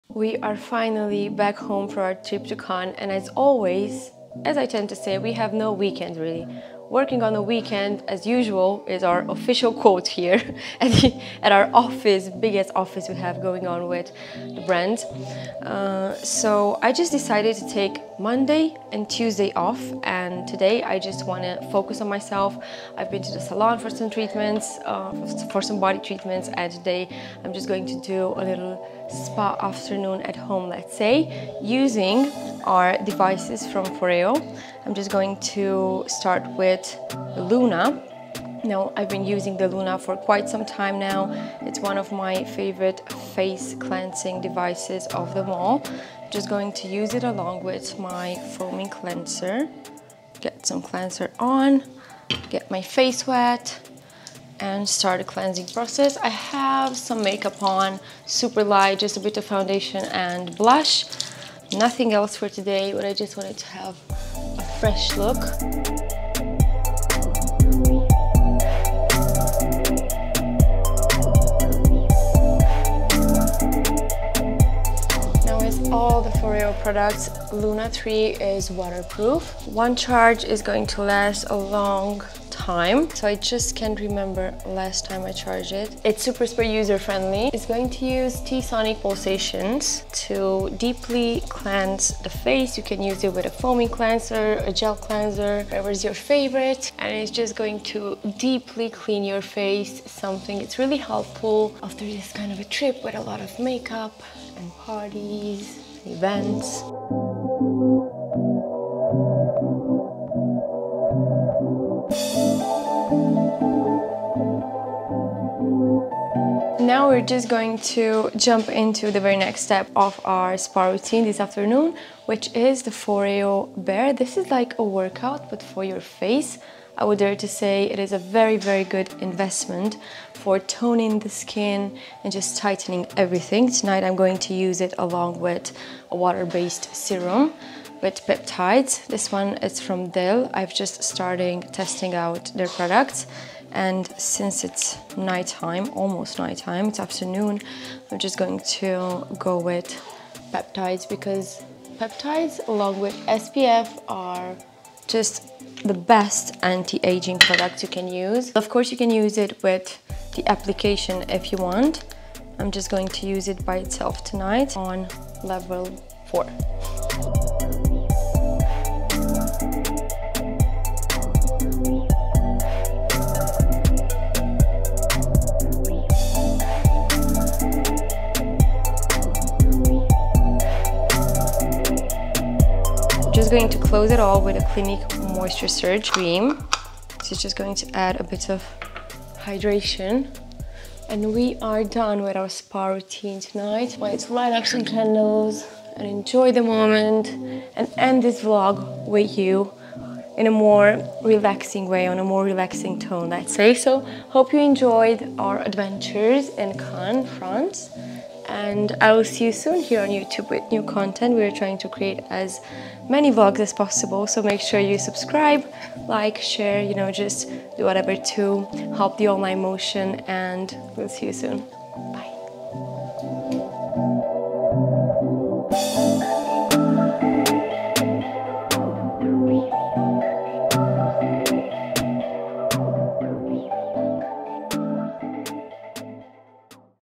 BEAUTY: SPA routine at home
Hey girls, we know you've been waiting and we have so many vlogs that we need to edit & post, so here is a lil AcBeautyMondays episode, we're back from Cannes (you'll see the timeline later) and off to new adventures, but first, a REFRESH, using FOREO devices, LUNA & Bear.

Love, A.